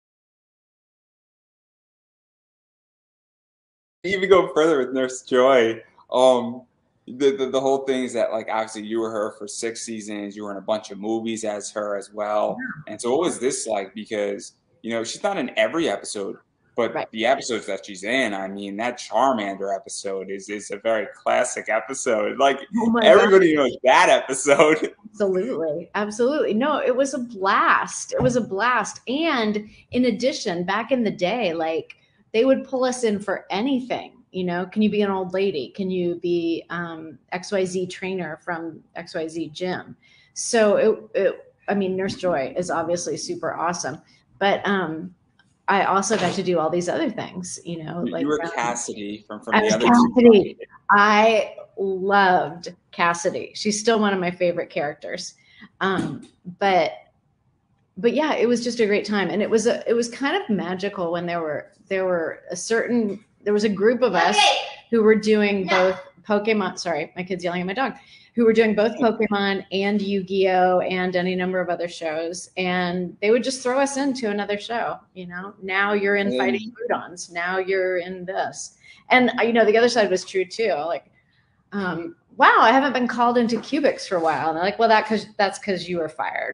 Being Thrown in and Out of Anime Voice Acting
This is a short clip from my full interview with Voice Actor Megan Hollingshead. In this clip Megan talks about how being thrown in and out of anime voice acting.